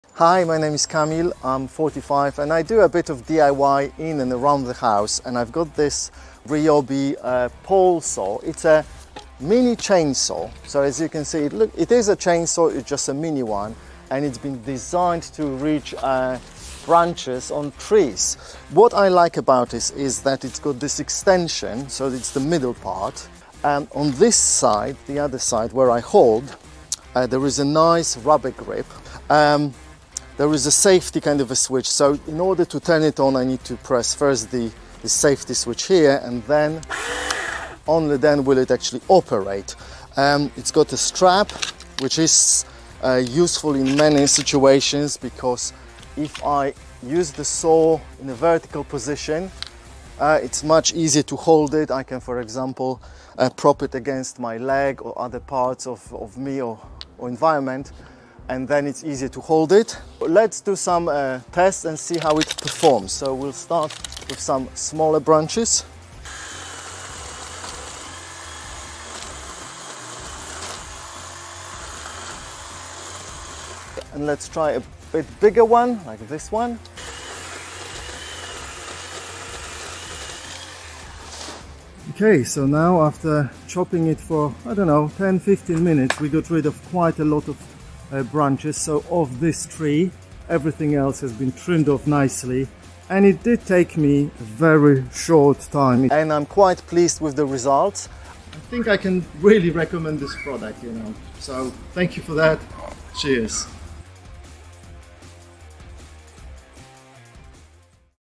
18V ONE+ Cordless 20cm Pole Saw Starter Kit [RPP182020]
18V ONE+ Cordless 20cm Pole Saw, recommended and tested by our Brand Ambassador.

Removes the need to use a ladder while trimming branches from trees and large bushes. Incorporating a high torque motor producing 5.5m/s chain speed combined with a 20cm Oregon bar and chain produces fast, clean cuts in overhead branches. The automatic chain bar lubrication mechanism and 30° angled cutting head gives the user full control and allows for easy pruning.